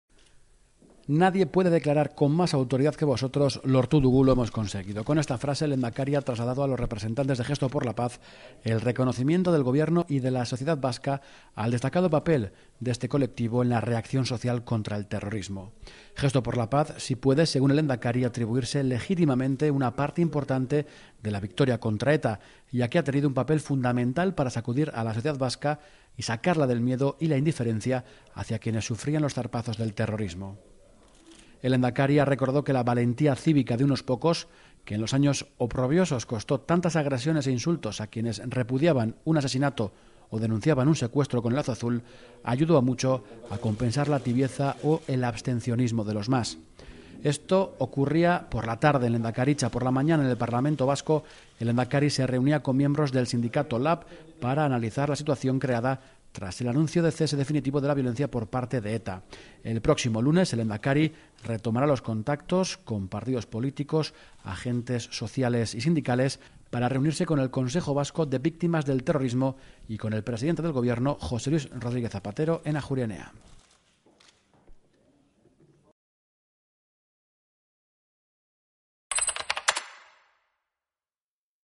El Lehendakari destaca el papel de Gesto por la Paz en la reacción social contra el terrorismo
El Lehendakari, Patxi López, se ha reunido esta tarde en la sede de Lehendakaritza con una representación de la Coordinadora Gesto por la Paz de Euskal Herria integrada por Fabián Laespada y Jesús Herreros, dentro de la ronda de contactos que está manteniendo con partidos políticos y organizaciones sociales tras el comunicado de ETA en el que anunció el cierre definitivo de sus actividades terroristas.